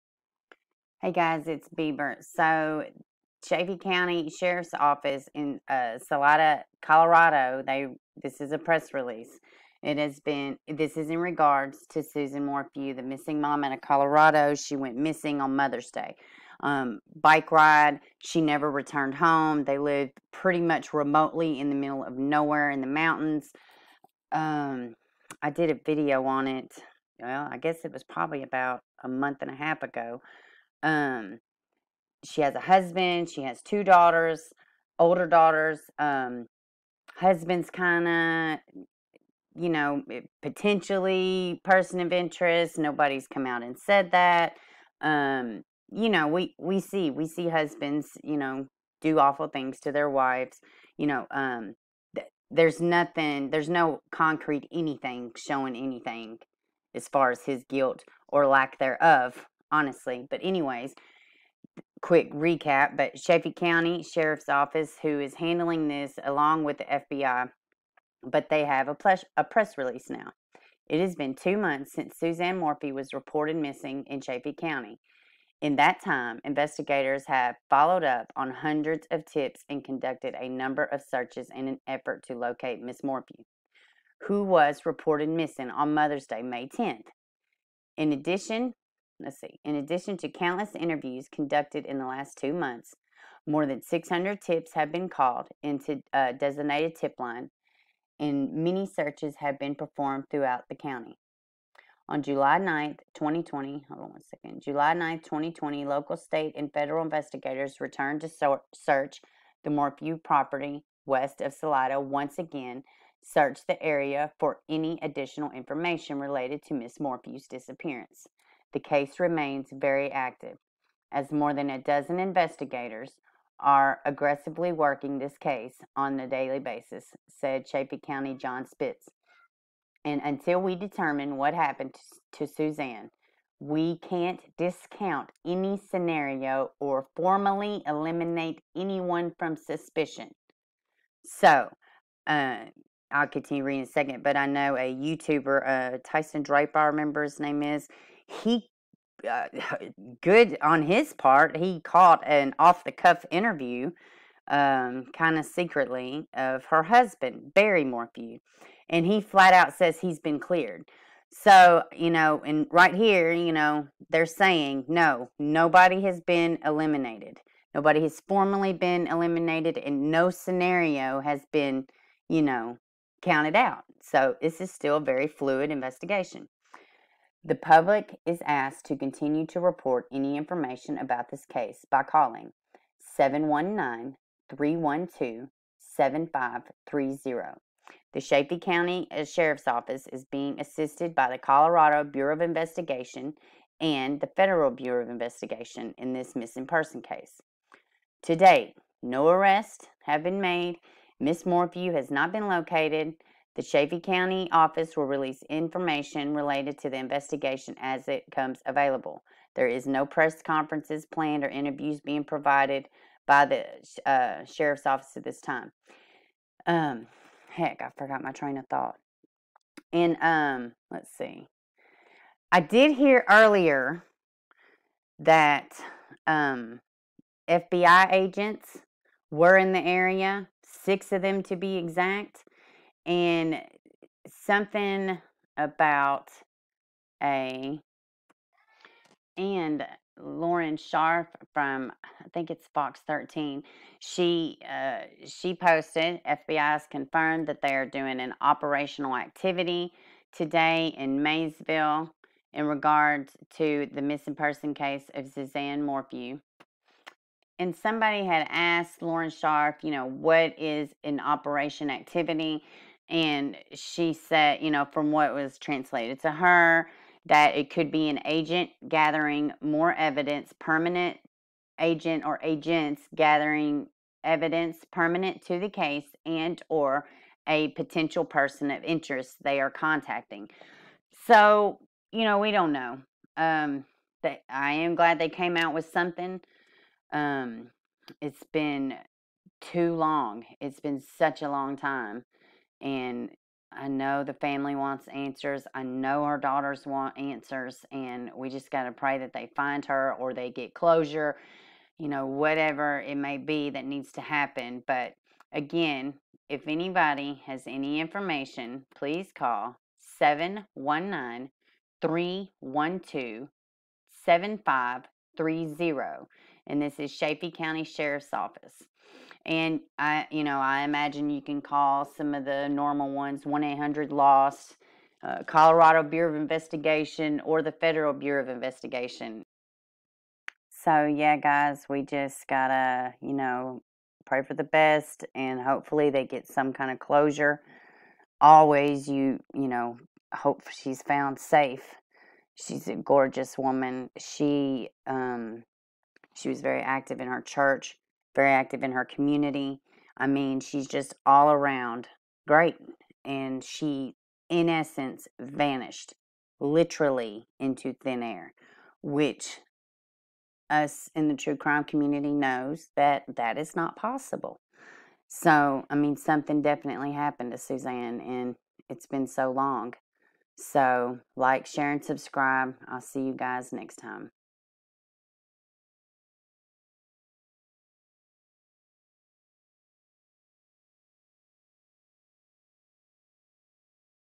SuzanneMorphew slight update ,press release from Chaffee County and another search at home
CHAFFEE COUNTY SHERIFF’S OFFICE
Salida, Colorado
PRESS RELEASE
It has been two months since Suzanne Morphew was reported missing in Chaffee County. In that time, investigators have followed-up on hundreds of tips and conducted a number of searches in an effort to locate Ms. Morphew, who was reported missing on Mother’s Day, May 10, 2020.
In addition to countless interviews conducted in the last two months, more than 600 tips have been called into a designated tip line, and many searches have been performed throughout the county.

On July 9, 2020, local, state, and federal investigators returned to search the Morphew property west of Salida to once again search the area for any additional information related to Ms. Morphew’s disappearance.
“This case remains very active, as more than a dozen investigators are aggressively working this case on a daily basis,” said Chaffee County Sheriff John Spezze. “And until we determine what happened to Suzanne, we can’t discount any scenario or formally eliminate anyone from suspicion.”
The Chaffee County Sheriff’s Office is being assisted by the Colorado Bureau of Investigation (CBI) and the Federal Bureau of Investigation (FBI) in this missing person case.
To date, no arrests have been made, and Ms. Morphew has not been located.